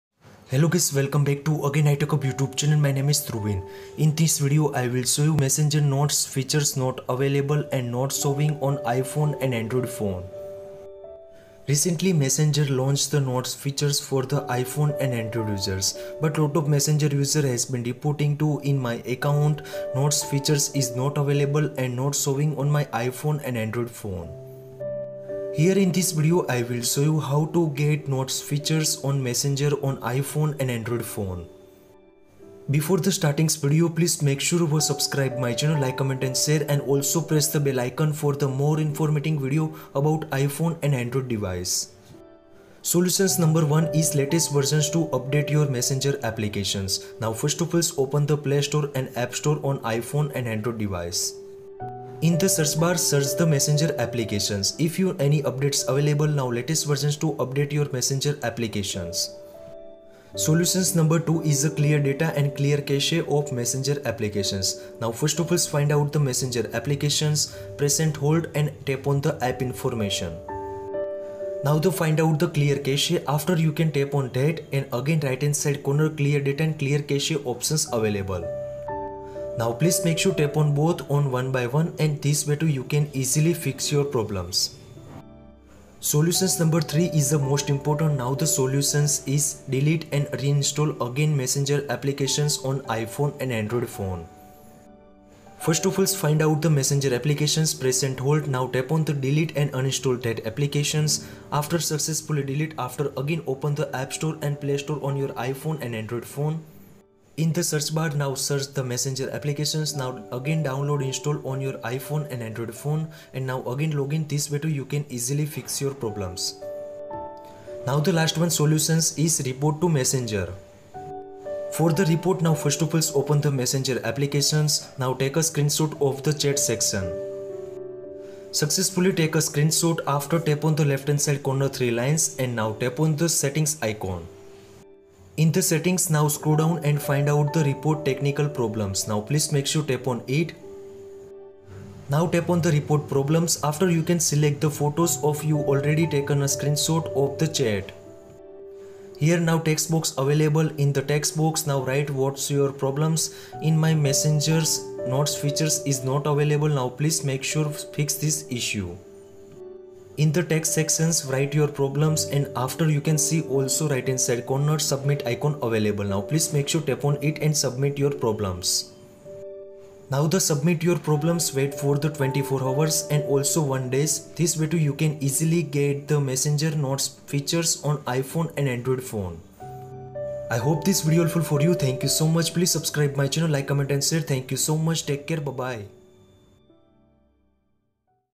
How to Fix Messenger Notes Not Showing (iPhone & Android)
Messenger Notes Features Not Showing (iPhone & Android)

Recently, messenger app lunched notes features for iphone & anroid users.

lot of messenger has been reported to messenger notes features not showing

how to fix messenger notes not working